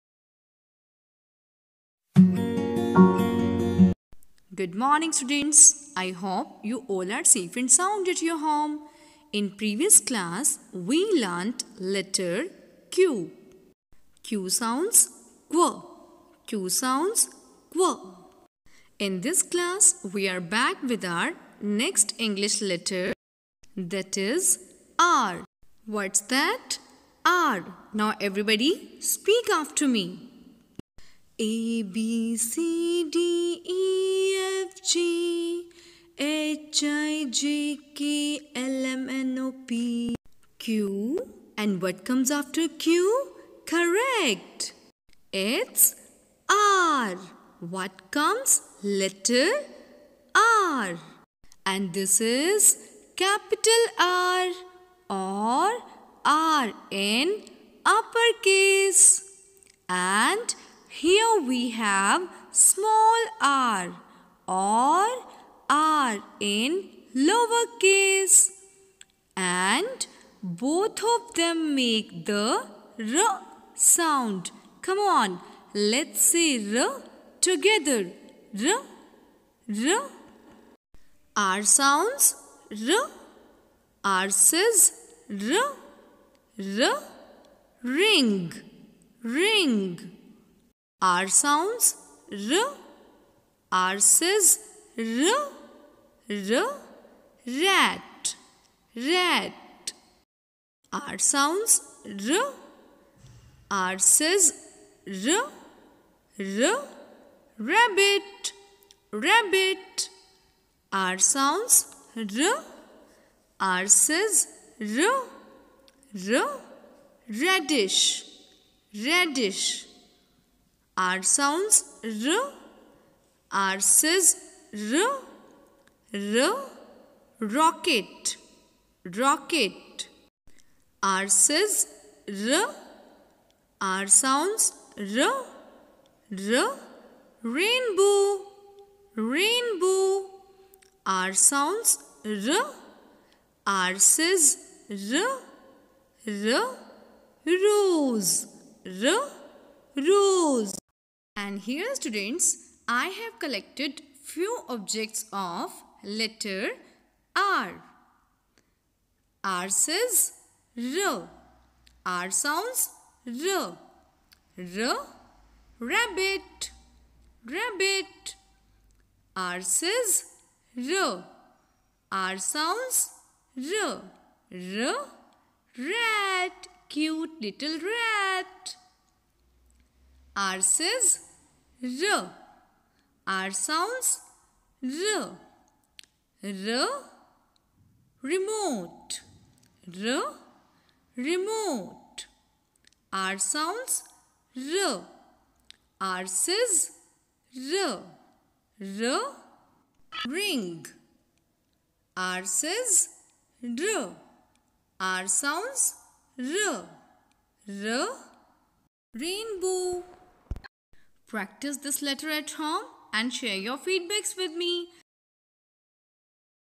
Language Development: English Introduction to letter 'R'
This video prepared by Agastya International School teacher helps the child to recognize the capital R and small r. Familiarize him/her with the /r/ sound the letter R makes.
Encourage and help child to look for objects starting with Letter R in the home.